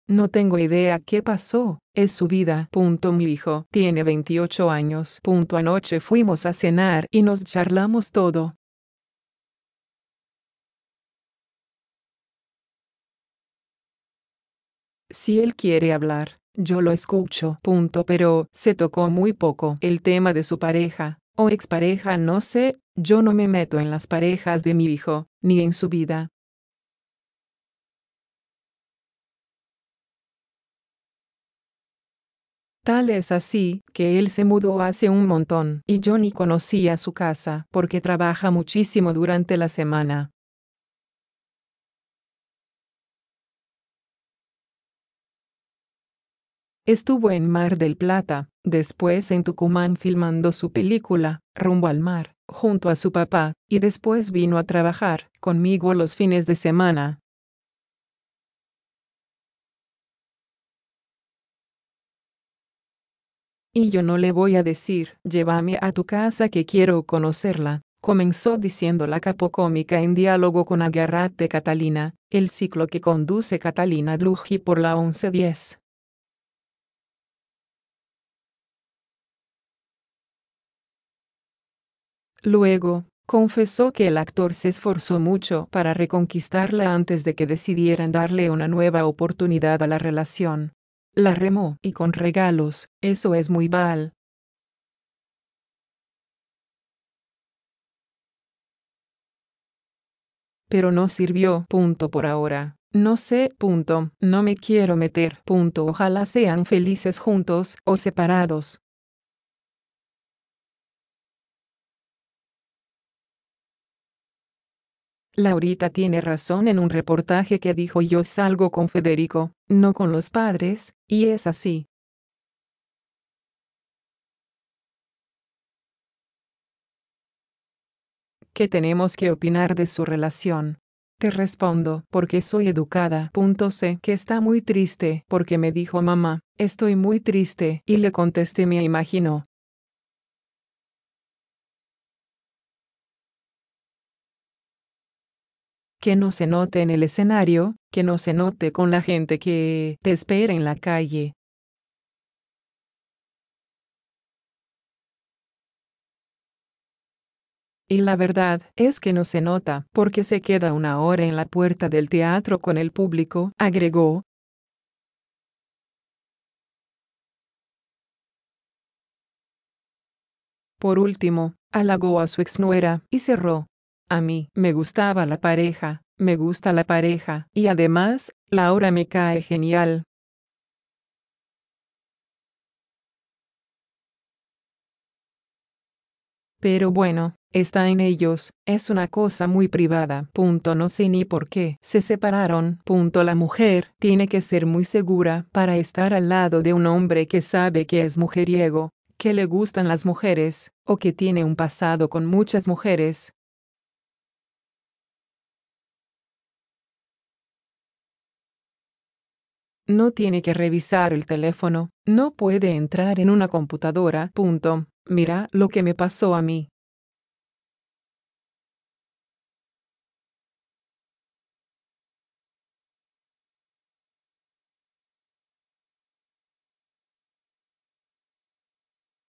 Carmen Barbieri, tras la separación de su hijo con Laurita Fernández
DUCK news-Carmen Barbieri, tras la separación de su hijo con Laurita Fernández: "Fede me dijo 'mamá, estoy muy triste'"

Con un diferencia de 24 horas, Federico Bal (28) y Laurita Fernández (27) confirmaron el final de su noviazgo. Y si bien prefirieron que los motivos que desencadenó la ruptura se mantenga en el ámbito de lo privado, Carmen Barbieri reveló una charla íntima que mantuvo con su hijo.

"No tengo idea qué pasó, es su vida. Mi hijo tiene 28 años. Anoche fuimos a cenar y nos charlamos todo. Si él quiere hablar, yo lo escucho. Pero se tocó muy poco el tema de su pareja, o expareja no sé, yo no me meto en las parejas de mi hijo, ni en su vida. Tal es así que él se mudó hace un montón y yo ni conocía su casa porque trabaja muchísimo durante la semana. Estuvo en Mar del Plata, después en Tucumán filmando su película (Rumbo al mar) junto a su papá, y después vino a trabajar conmigo los fines de semana. Y yo no le voy a decir 'llevame a tu casa que quiero conocerla'", comenzó diciendo la capocómica en diálogo con Agarrate Catalina, el ciclo que conduce Catalina Dlugi por La Once Diez.
Luego, confesó que el actor se esforzó mucho para reconquistarla antes de que decidieran darle una nueva oportunidad a la relación: "La remó y con regalos, eso es muy Bal... pero no sirvió. Por ahora, no sé... no me quiero meter. Ojalá sean felices juntos o separados. Laurita tiene razón en un reportaje que dijo 'yo salgo con Federico, no con los padres', y es así. ¿Qué tenemos que opinar de su relación? Te respondo porque soy educada".

"Sé que está muy triste porque me dijo 'mamá, estoy muy triste' y le contesté 'me imagino. Que no se note en el escenario, que no se note con la gente que te espera en la calle'. Y la verdad es que no se nota porque se queda una hora en la puerta del teatro con el público", agregó.
Por último, halagó a su exnuera y cerró: "A mí me gustaba la pareja, me gusta la pareja y además, Laura me cae genial. Pero bueno, está en ellos, es una cosa muy privada. No sé ni por qué se separaron. La mujer tiene que ser muy segura para estar al lado de un hombre que sabe que es mujeriego, que le gustan las mujeres, o que tiene un pasado con muchas mujeres. No tiene que revisar el teléfono, no puede entrar en una computadora... ¡mirá lo que me pasó a mí!".

PRONTO
DUCKnews